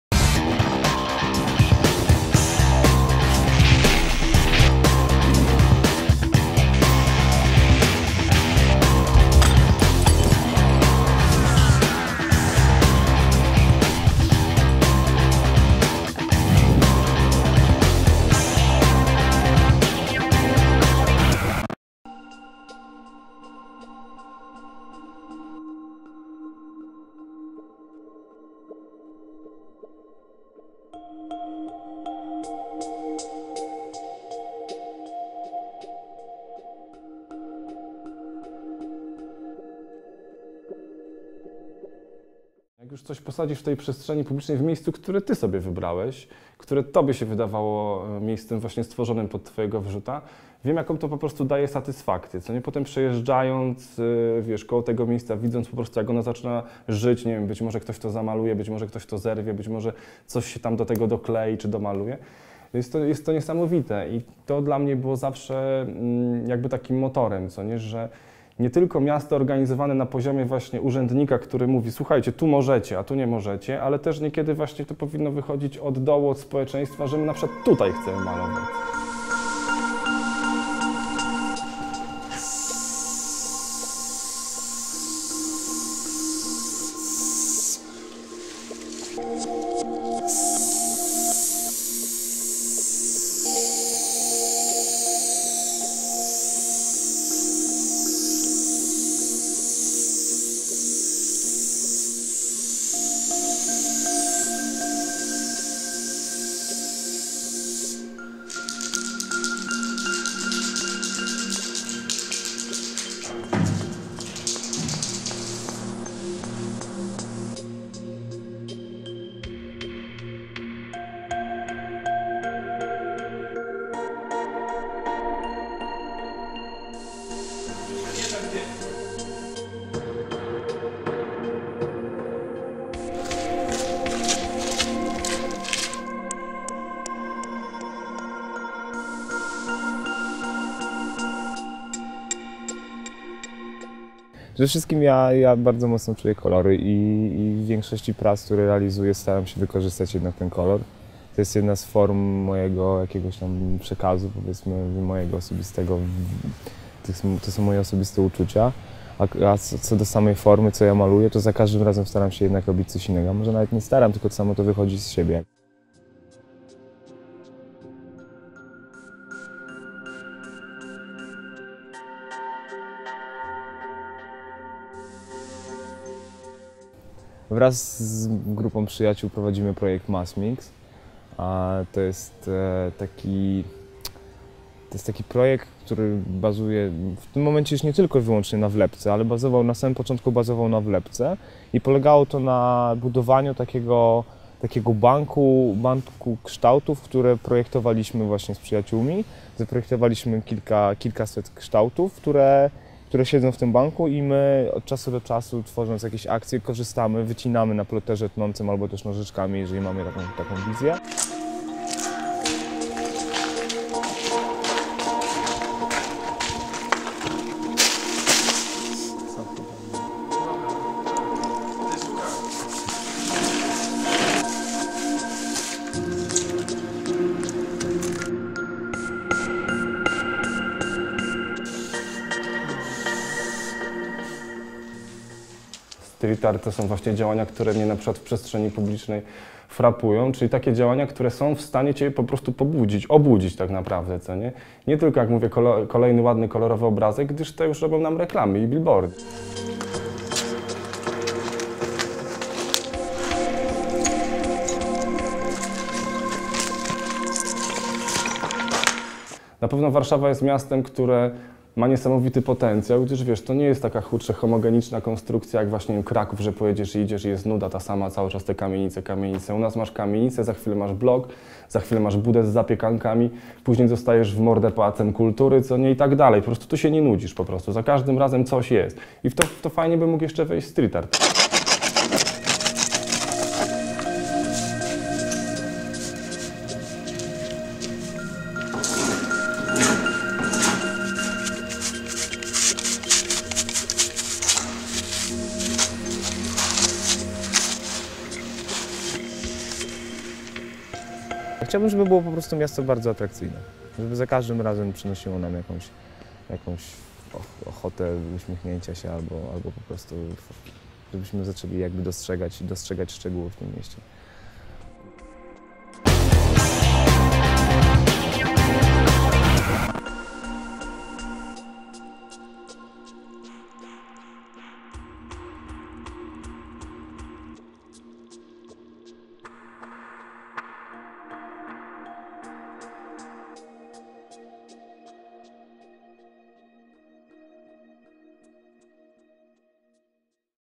War-szał: Jak street art otwiera miasto
Vlepki, murale, graffiti... Czym jest street art? Przestał być postrzegany jako wandalizm, a wręcz coraz odważniej wkracza do galerii. My patrzymy na scenę street artową krytycznym okiem postaci skupionych wokół vlep[v]netu i V9